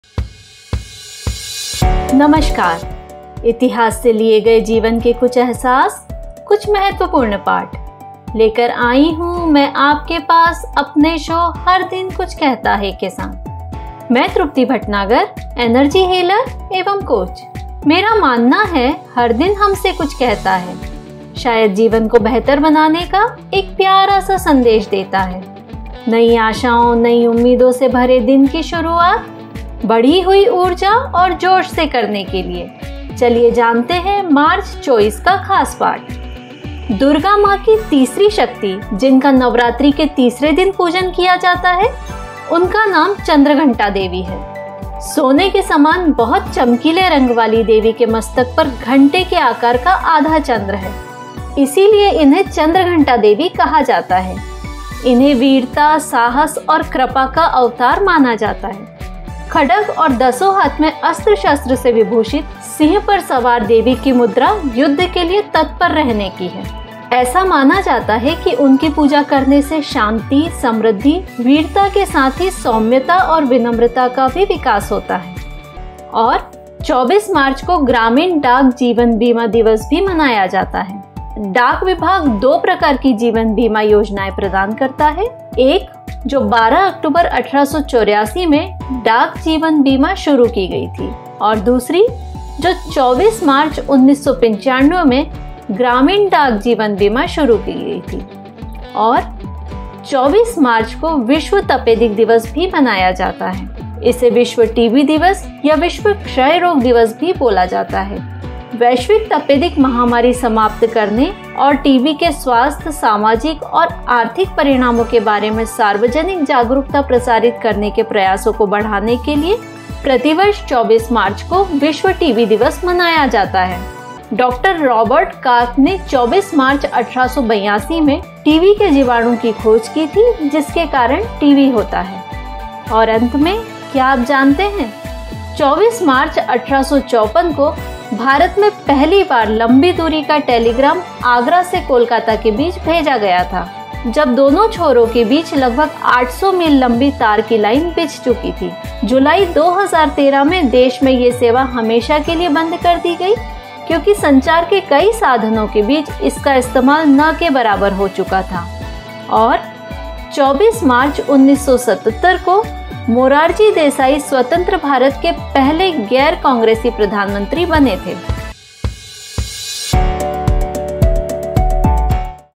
Chandraghanta Devi Third Day of Navratri
Each day of Navratri is dedicated to one of the nine forms of Goddess Durga. The Third day of Navratri is dedicated to Goddess Chandraghanta, the third form of Goddess Durga.

Navratri is a significant festival for Hindus, and the nine-day-long celebration marks the victory of good over evil. Each day is dedicated to the worship of a different form of Goddess Durga, and Chandraghanta Devi is one of the important forms of the goddess that is worshipped during this time.

In this video/podcast we’ll walk you through:
Messages of today mean 24th of March
Goddess Chandraghanta
Rural ​​​​​​​​​​​​​​​​​​​​​​​​​​​​​​​​​​Postal Life Insurance
World Tuberculosis (TB) Day
Conclude

TIMESTAMPS
0:00 Intro
0:35 Goddess Chandraghanta
1:15 Rural ​​​​​​​​​​​​​​​​​​​​​​​​​​​​​​​​​​Postal Life Insurance
1:39 World Tuberculosis (TB) Day
2:14 Other Information